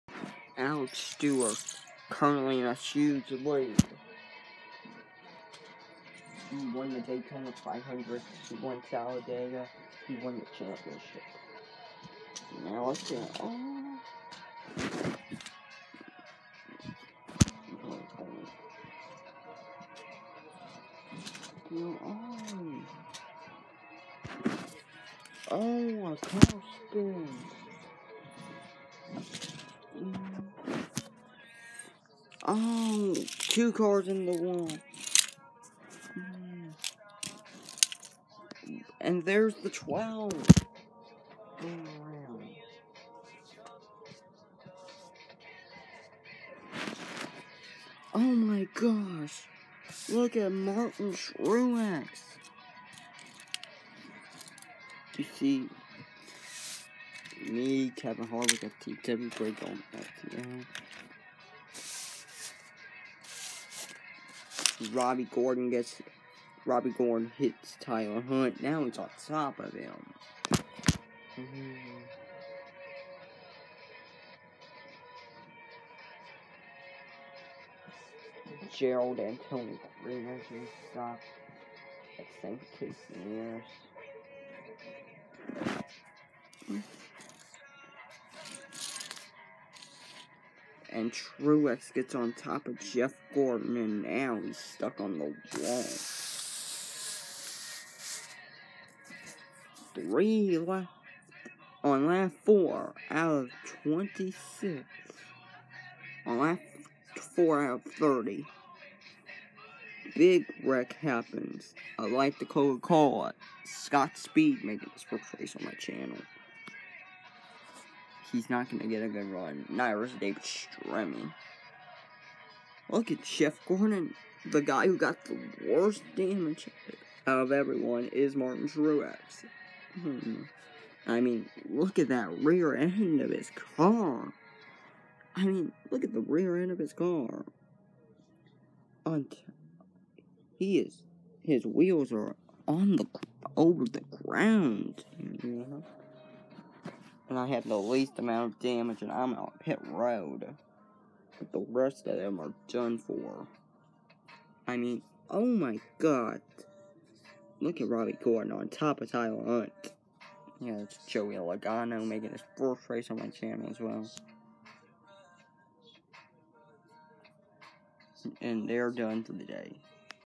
red flag for big wreck
Worst damage went to truex
Jeff the pelican went to garage off camera due to start and parkers